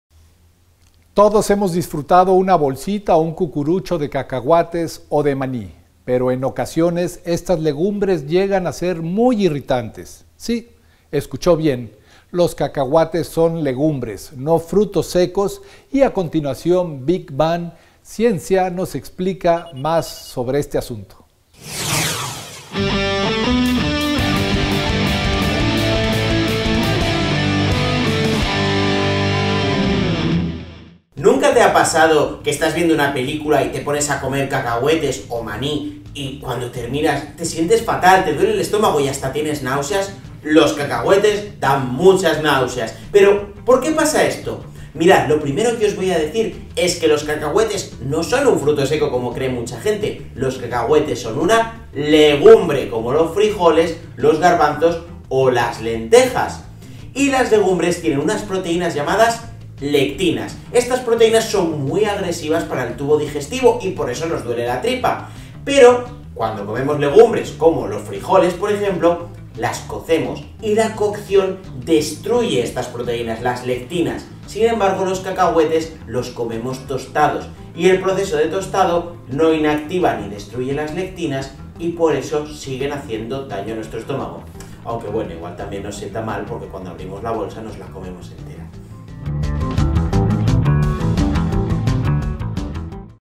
¿Por qué dan náuseas después de comer cacahuates?
¿Al­gu­na vez te ha pa­sa­do que des­pués de co­mer ca­cahua­tes te dan náu­seas? Esto pasa por­que los ca­cahua­tes no son un fru­to seco como mu­chas per­so­nas pien­san, son le­gum­bres como los fri­jo­les, los gar­ban­zos o las len­te­jas.